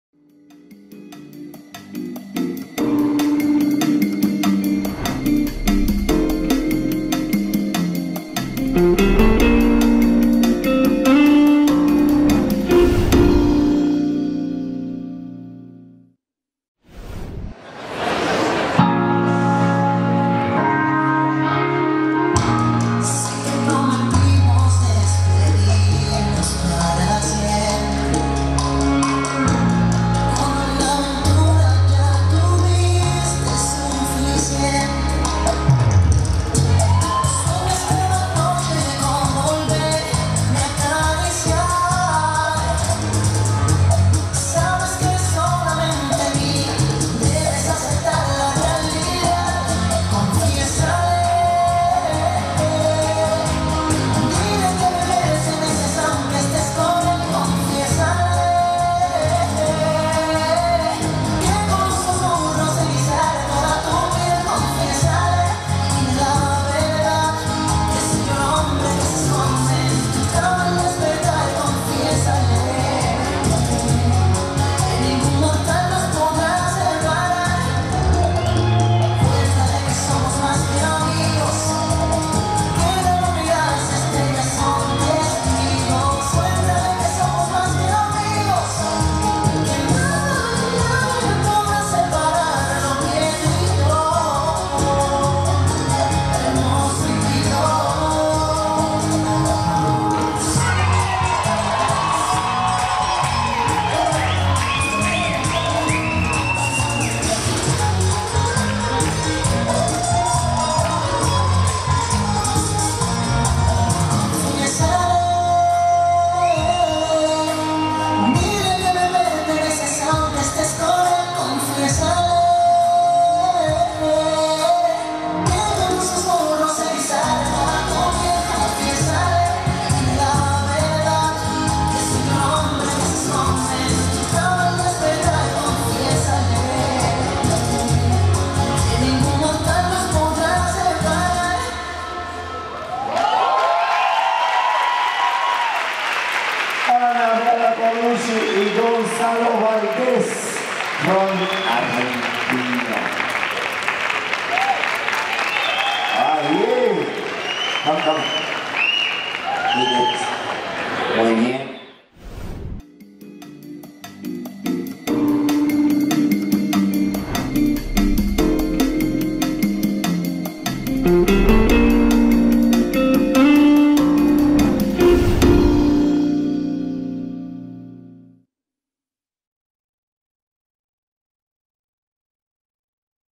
Baila Mundo - Anabella Paulicci e Gonzalo Valdes (Brasil Latin Open 2018)
Confiram apresentação do casal Anabella Paulicci e Gonzalo Valdes da categoria Bachata Sul-americana do Brasil Latin Open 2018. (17/06/2018)
Check out the presentation of the couple Anabella Paulicci and Gonzalo Valdes from the category Bachata South American at Brazil Latin Open 2018.
Vea la presentación de la pareja Anabella Paulicci y Gonzalo Valdes de la categoría Bachata Sudamericana en el Brazil Latin Open 2018.